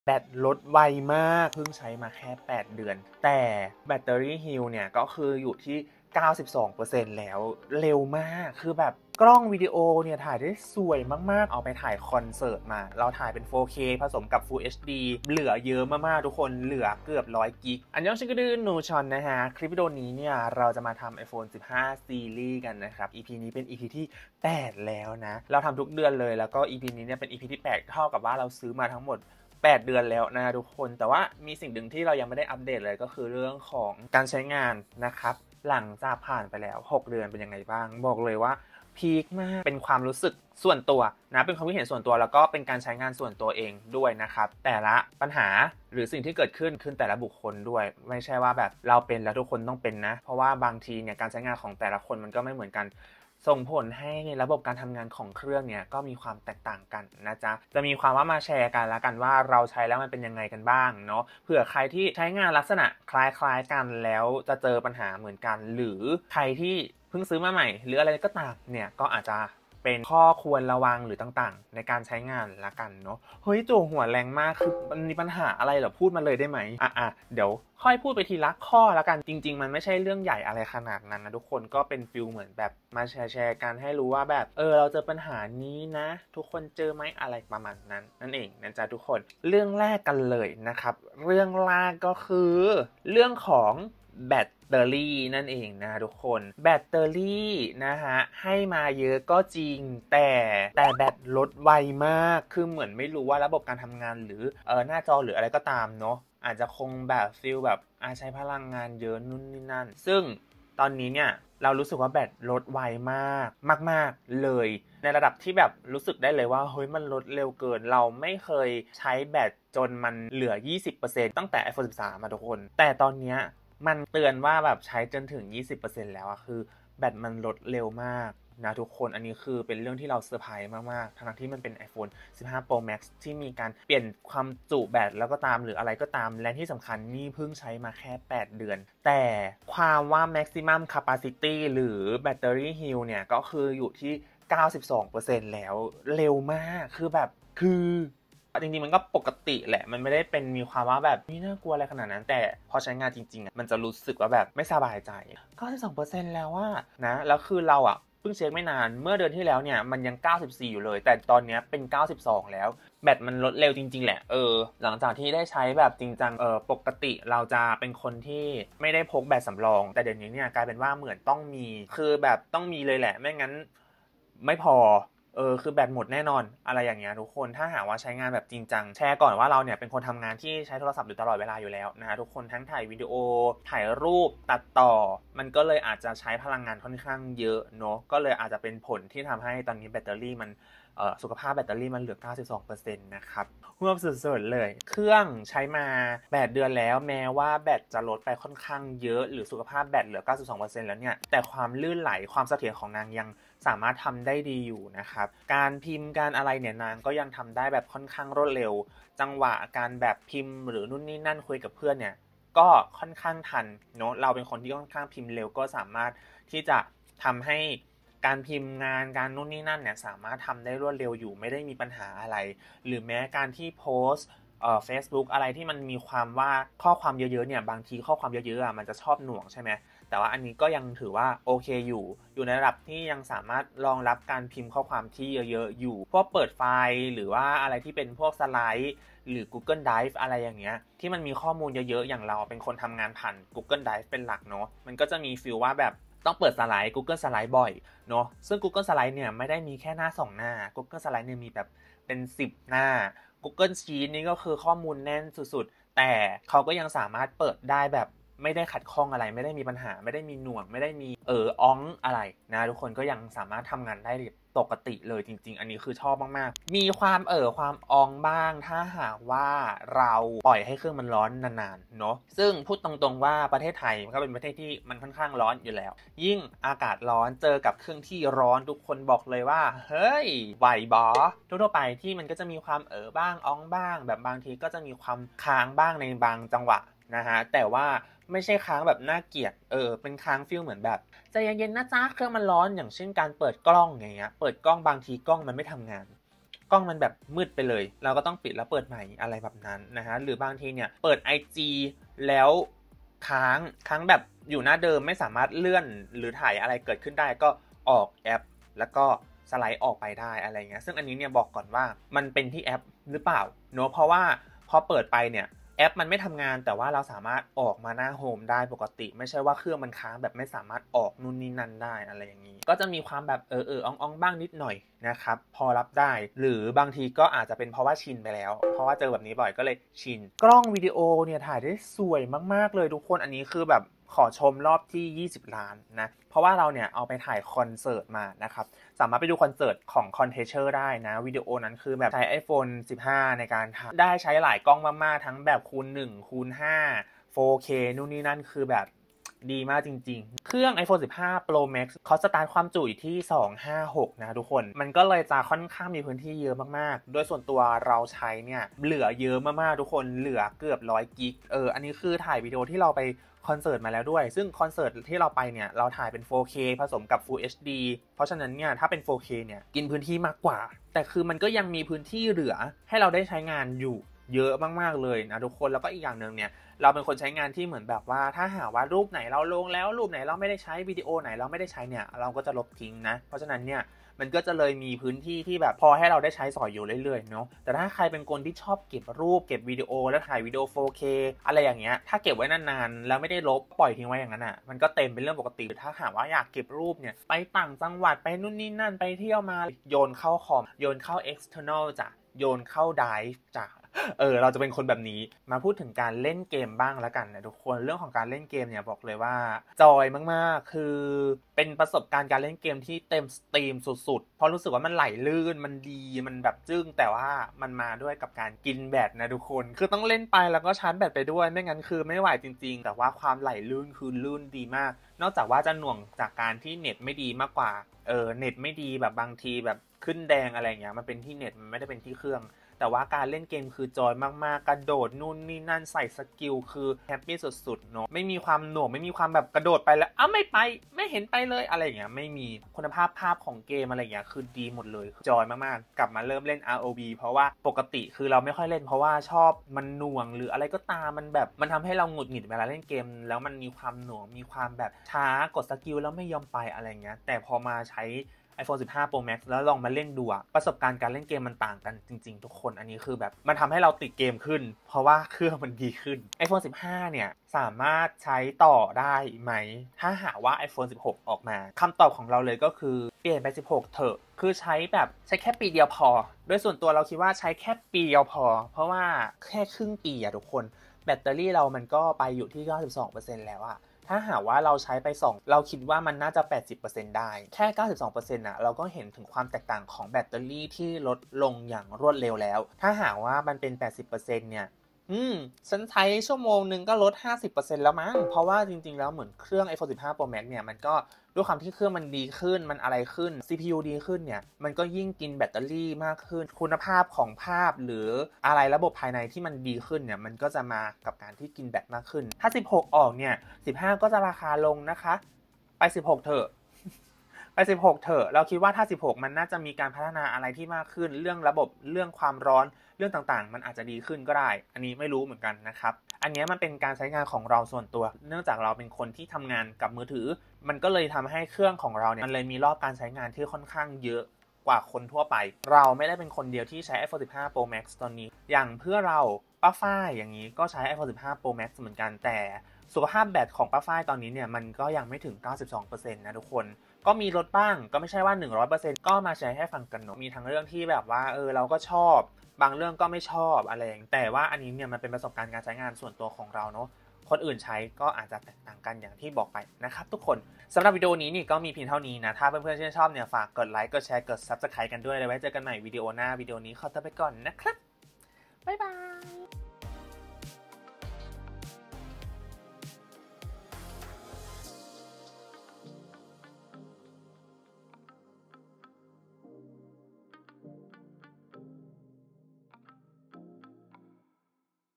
Iphone 15 series ep8 📱💬 รีวิว อัปเดต ใช้ไอโฟน 15 มาครึ่งปีแล้ววว !! | nusyeon
มาอัปเดตกันหน่อยยย ใช้ IPhone 15 มาครึ่งปี มีอะไรเปลี่ยนแปลงบ้าง ? น่าใช้อยู่ไหม หรือควรรอ 16 ดี คลิปนี้มาแชร์ความเห็นกันค้าบบ